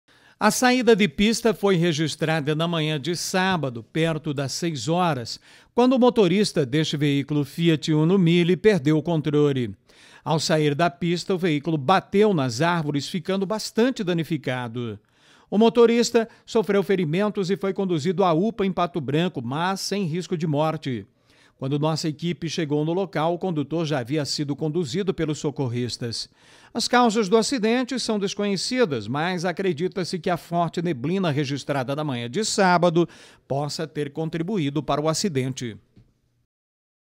Acidente na PR-469
Acidente: Motorista perde o controle e sai da pista;